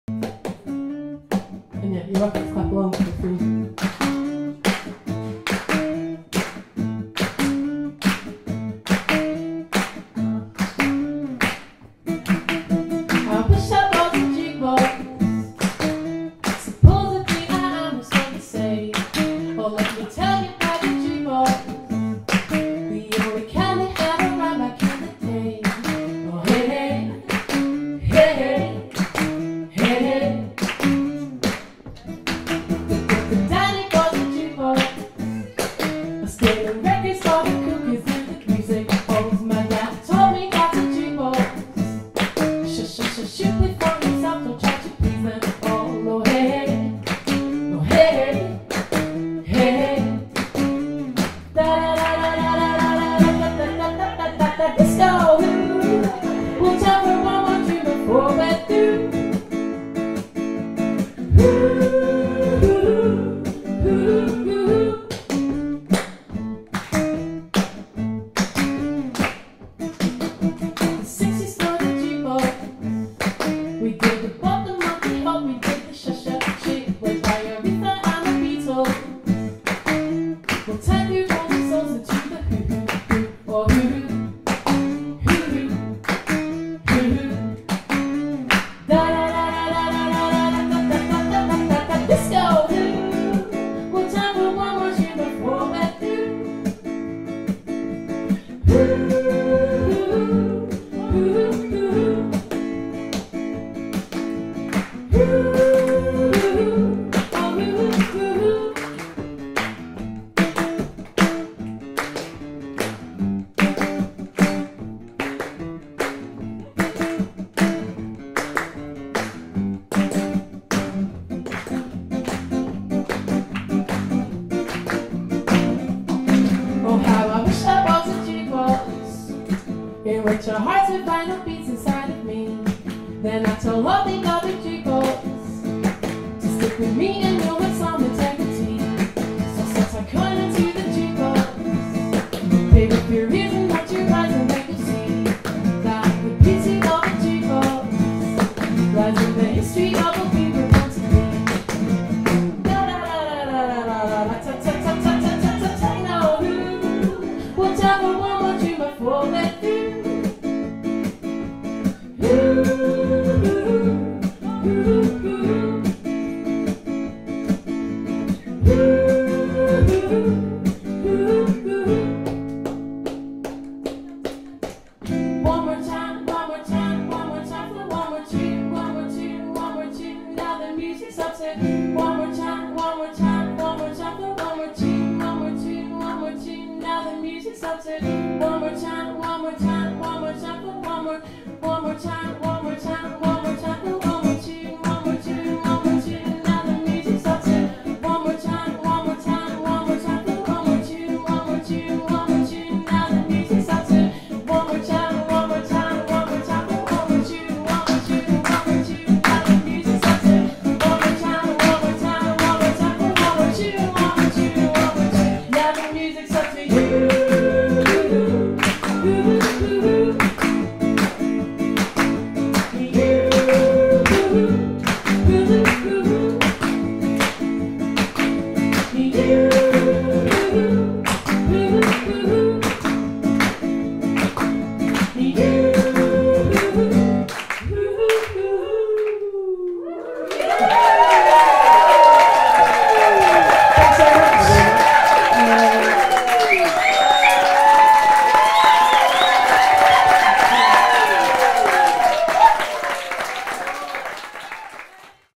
The Banana Sessions Jukebox
The Banana Sessions Songwriters Night at The House Concerts @42, Friday 14th September 2012